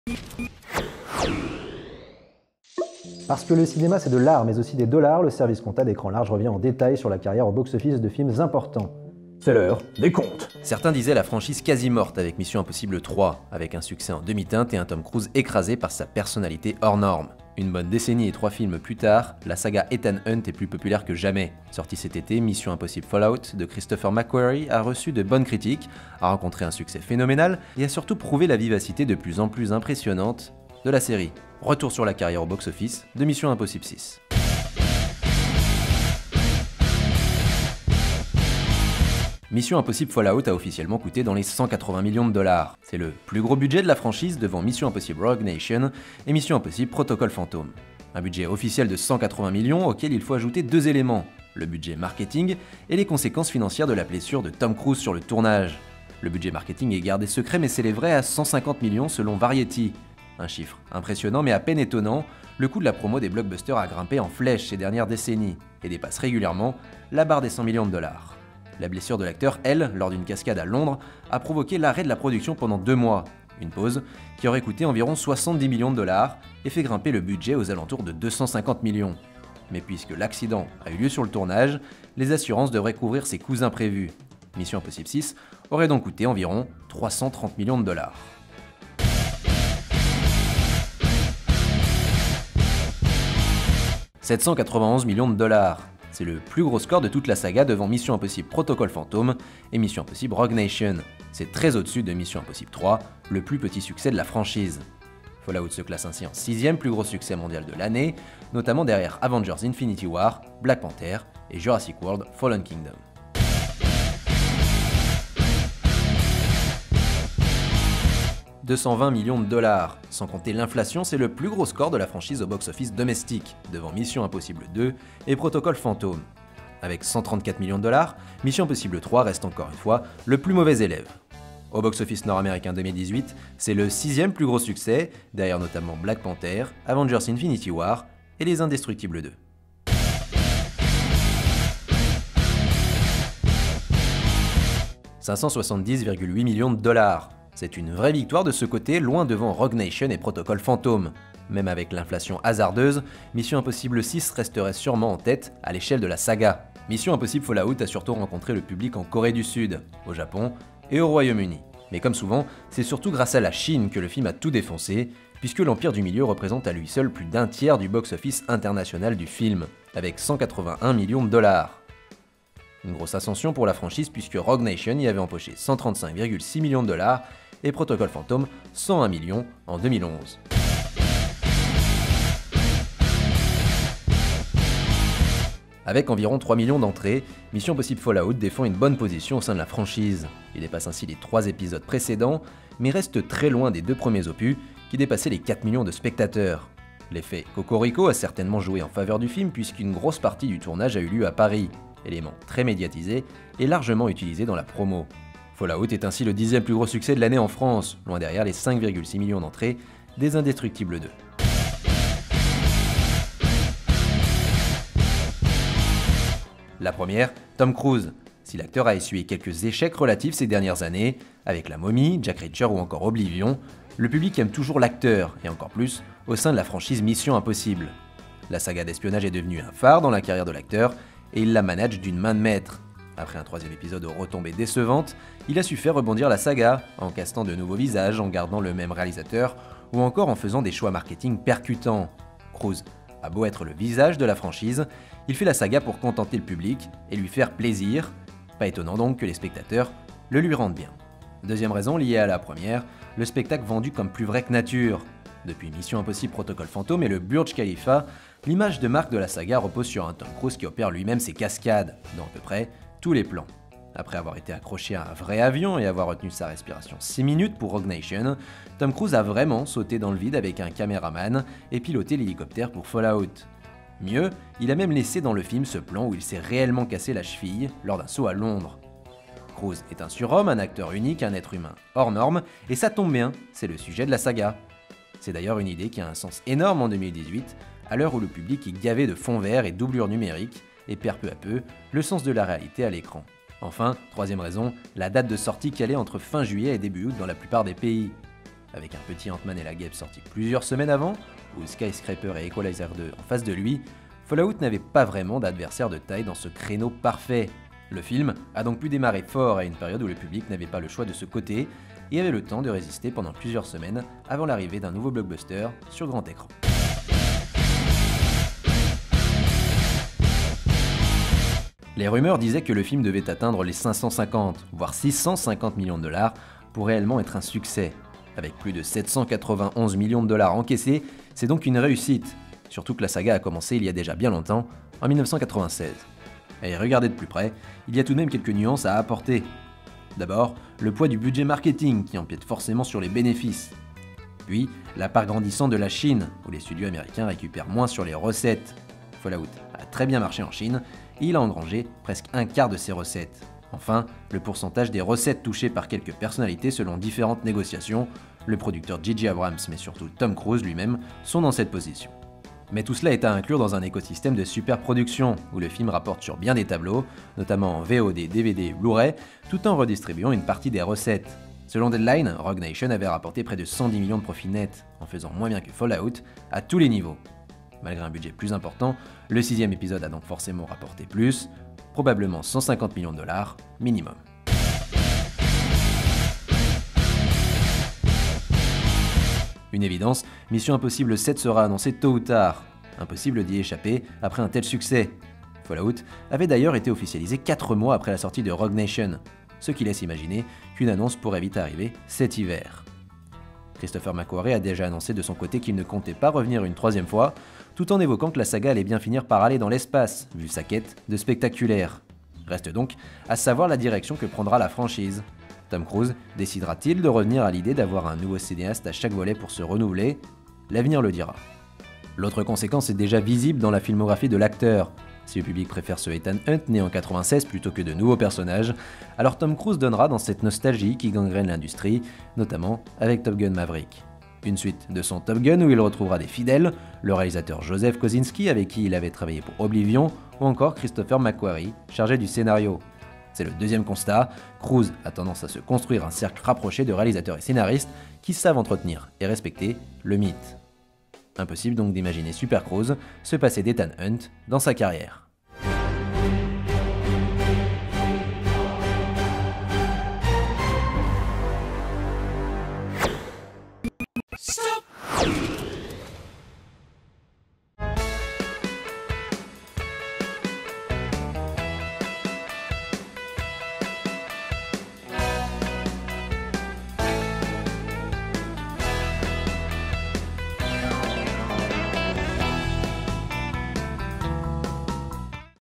MISSION IMPOSSIBLE - FALLOUT — L'HEURE DES COMPTES
A l'occasion de sa sortie en DVD et Blu-ray le mercredi 5 décembre, on revient sur le succès phénoménal de Mission Impossible : Fallout au cinéma...

Combien ça a coûté ? Combien ça a rapporté ? C'est l'heure des comptes !

Avec EcranLarge.com, retrouvez toute l'actualité cinéma, séries, DVD/Blu-Ray et Jeux Vidéo.